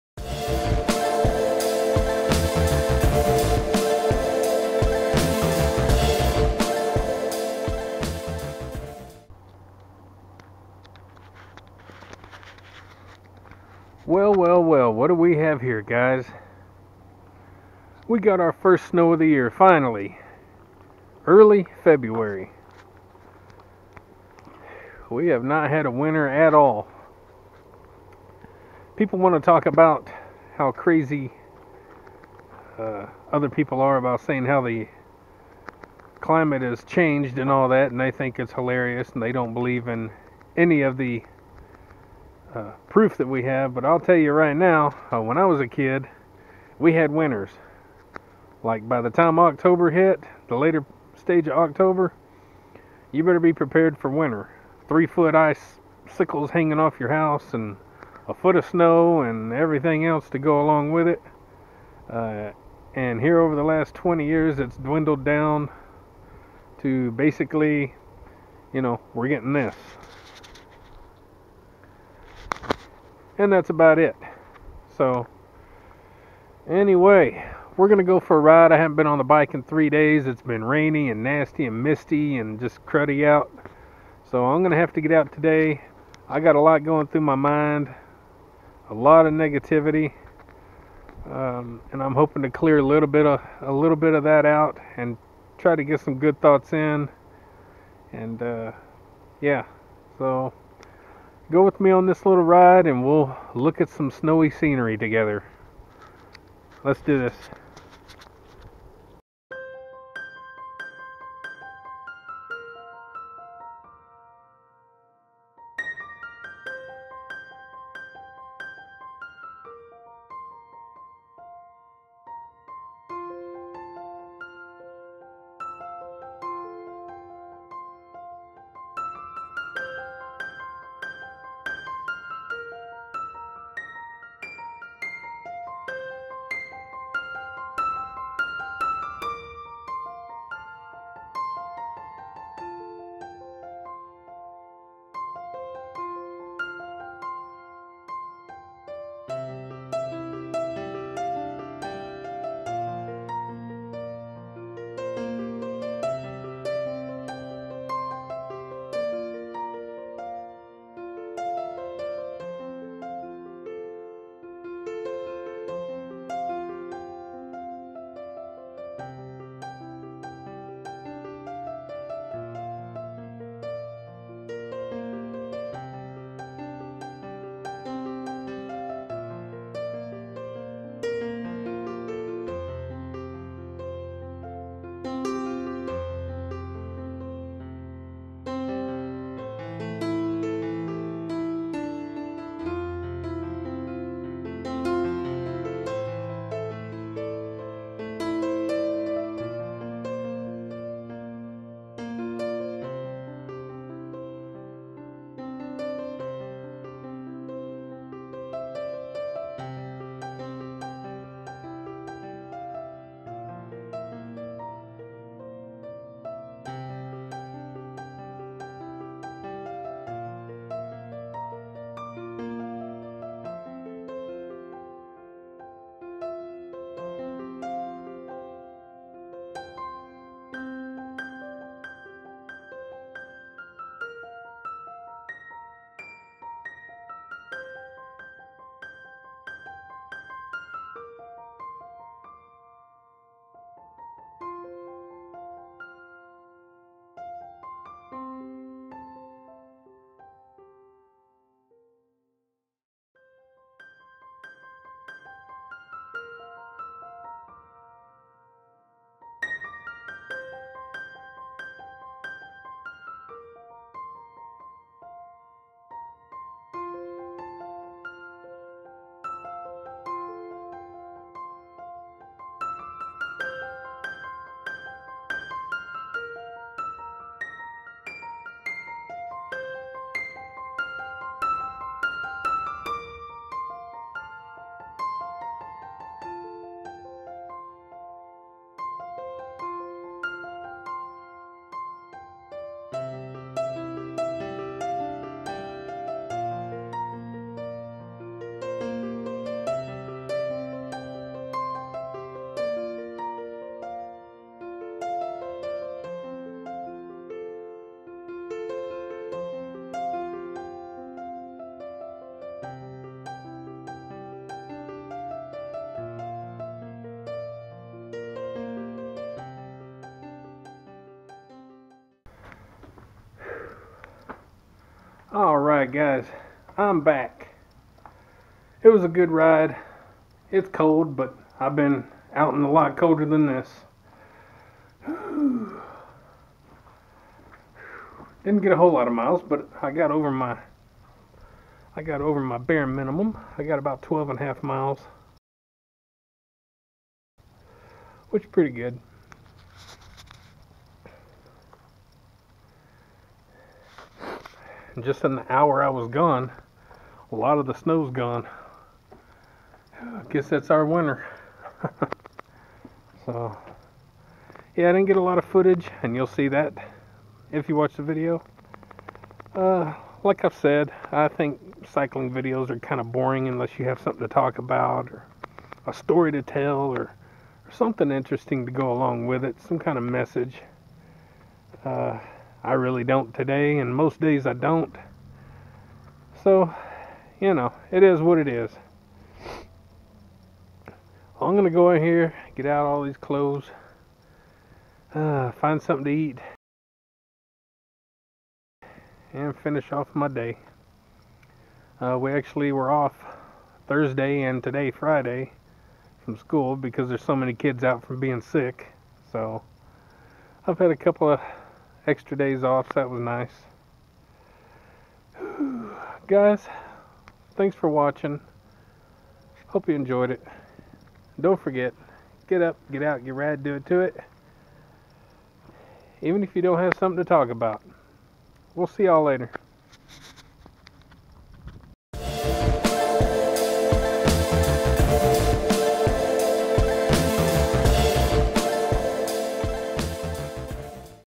Winter Raleigh Redux Ride With SNOW! 2 7 20
The rain has moved off but the snow moved in.Lets see if we can get in a few miles and enjoy the day. GET UP,GET OUT,GET RAD and DO IT TO IT!!